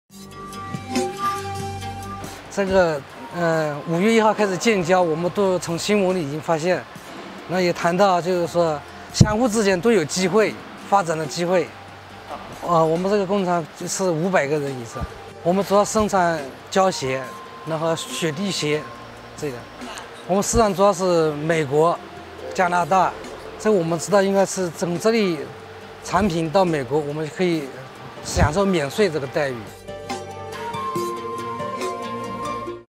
Empresa capital chino, DJM Footwear, se instala en RD optimista con nueva era diplomática
TAMBORIL, SANTIAGO.- Con gran regocijo, Lai Chang Fu, gerente de desarrollo y tecnología de DJM Footwear, consideró que las relaciones diplomáticas entre República Dominicana y la República Popular de China, brindarán más oportunidades para inversionistas del país asiático.

DJM Footwear es una empresa de calzados de capital chino que está siendo instalada en el Parque Zona Franca Industrial Tamboril, en un área de de 183 mil pies cuadrados, y que próximamente será inaugurada. Generará alrededor de 500 empleos.

Lai Chang Fu manifestó su confianza en esta nueva era diplomática y estimó que llegarán más inversionistas al país.

De igual modo, resaltó que estas nuevas relaciones facilitarán el traslado desde y hacia China.

Entre las motivaciones para instalarse en el país, destacó el excelente clima de inversiones.

República Dominicana se ha convertido en una potencia regional en la industria del calzado.

La producción de calzado ha tenido un gran impacto en la provincia Santiago en términos económicos y en generación de empleos.